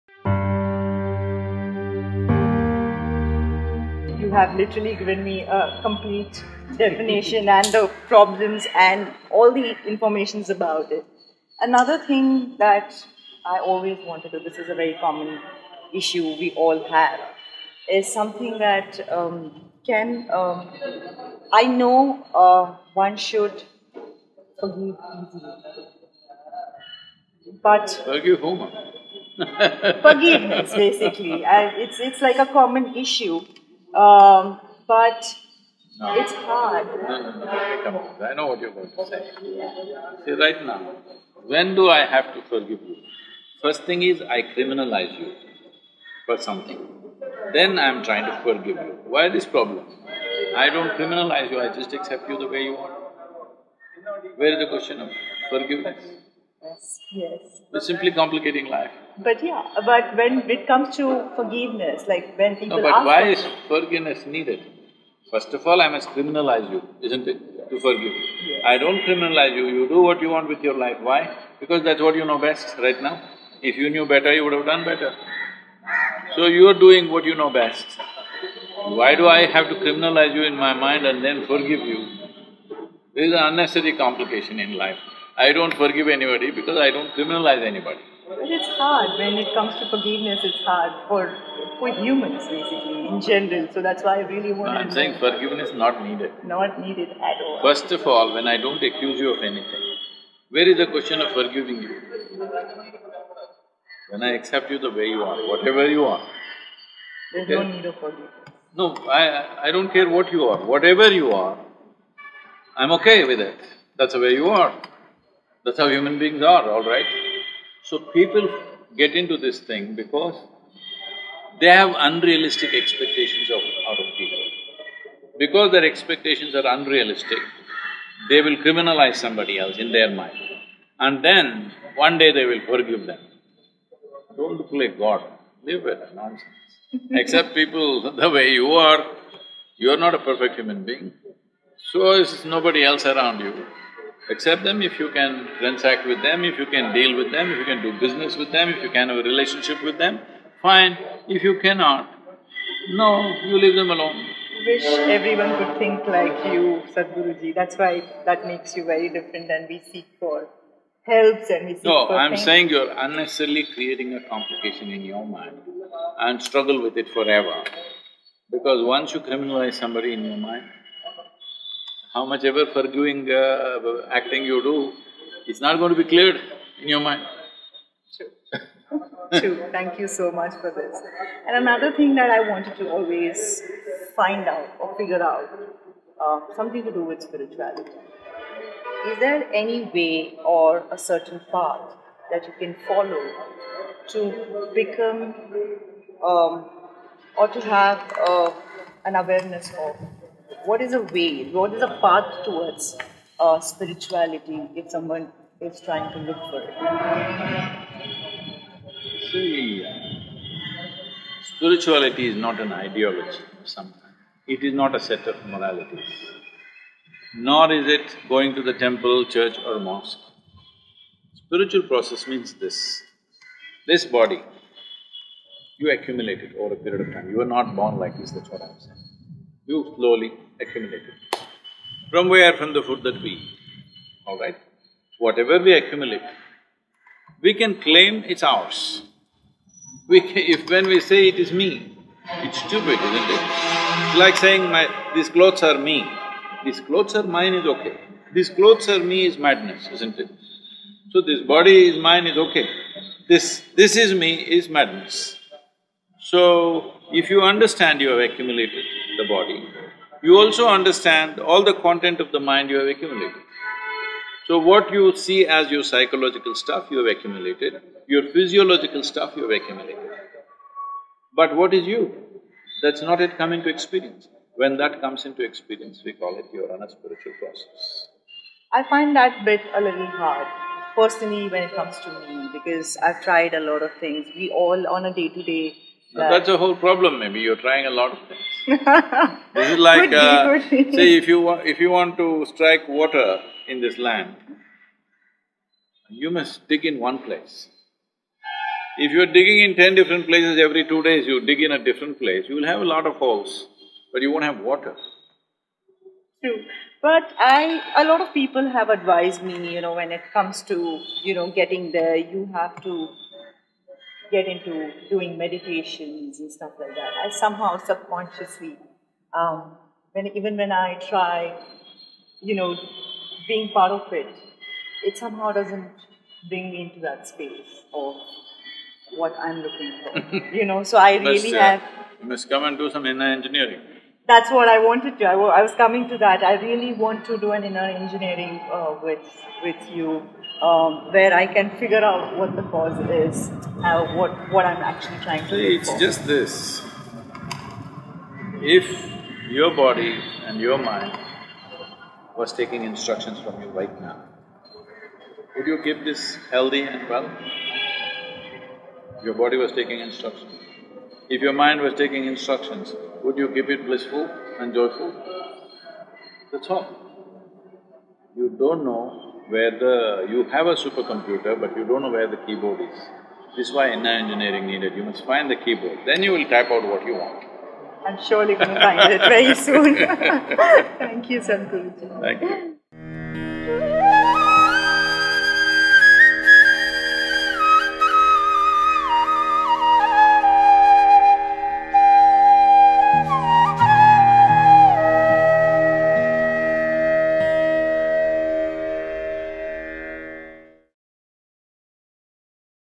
How to Forgive Someone? Raai Laxmi Asks Sadhguru
Raai Laxmi asks Sadhguru about how to forgive someone and shares how hard it is to actually do this. The highly engaging discussion finally heads into how one actually becomes spiritual?